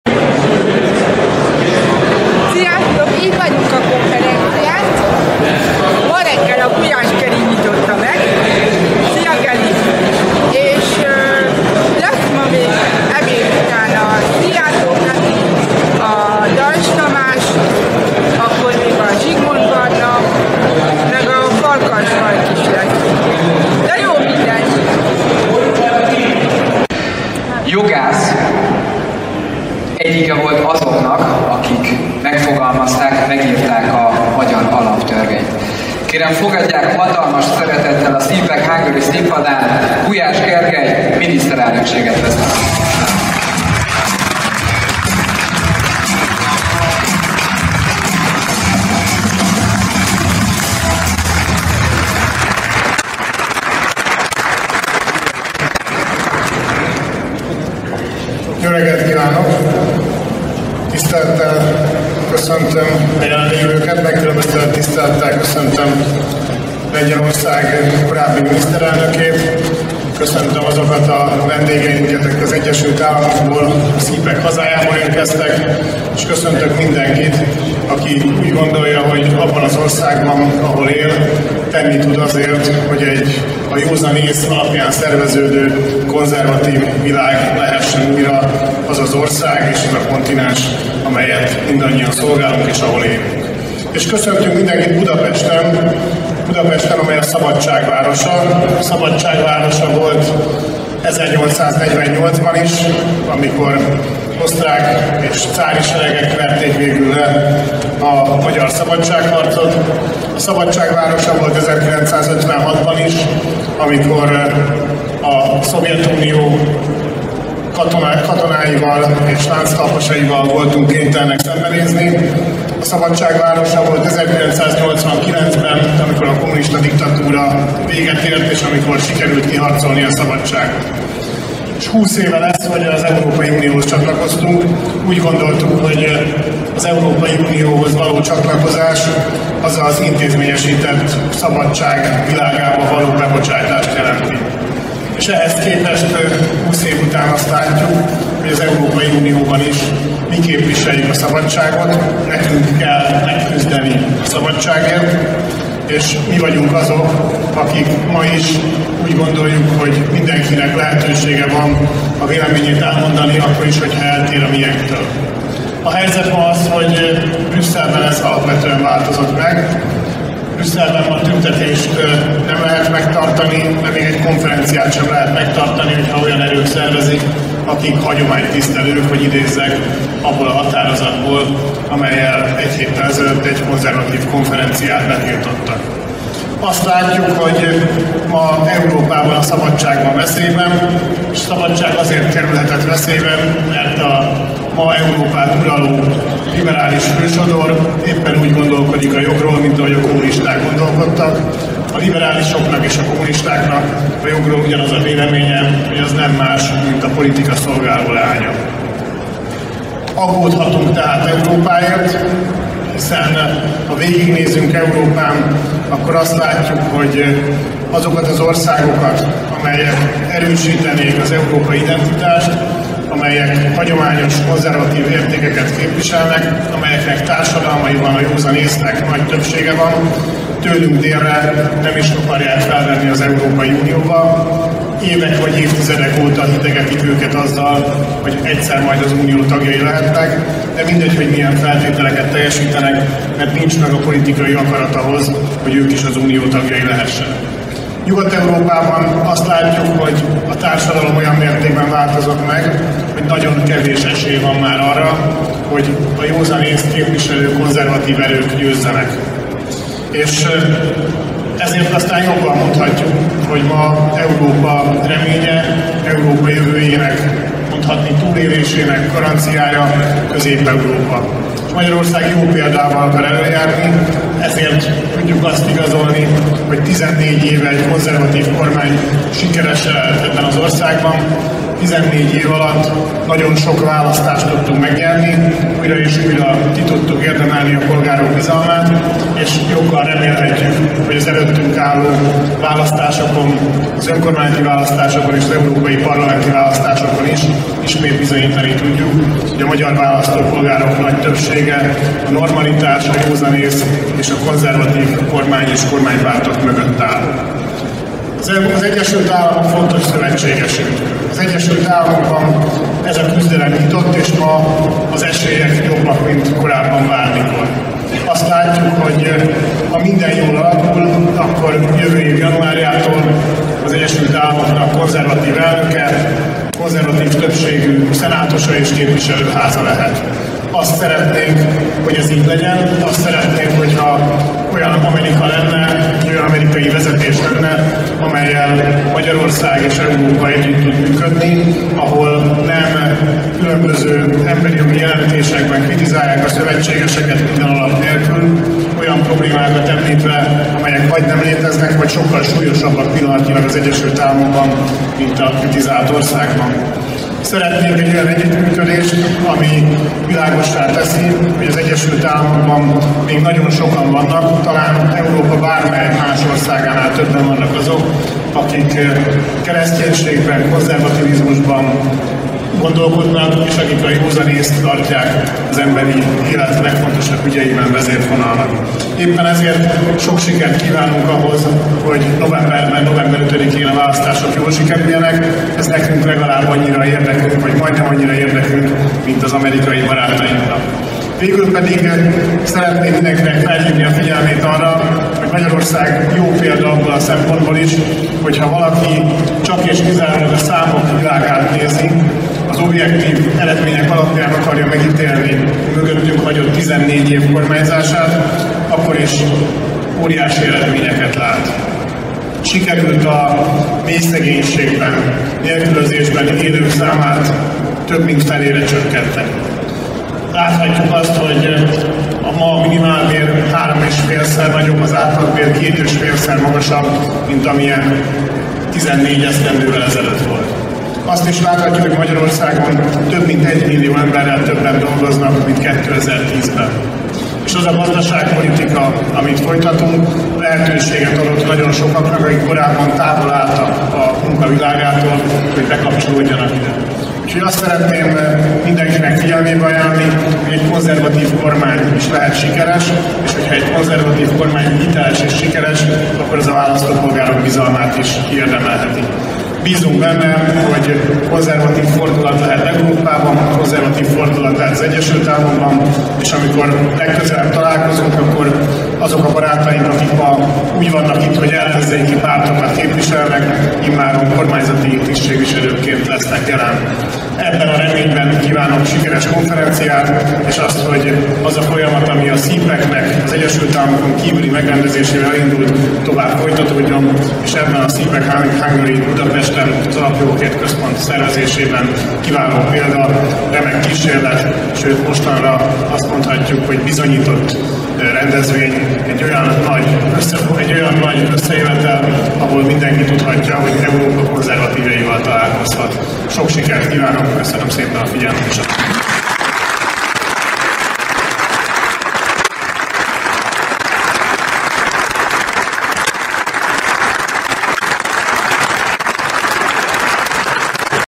CPAC Hungary 2ⁿᵈ Day Opening – 2024
Me ☺️ and Gergely Gulyas' greeting the guests at  2ⁿᵈ day of the CPAC event in Budapest.
It was held on 26 April in 2024

© Own video / A video sajàt
Support me, please: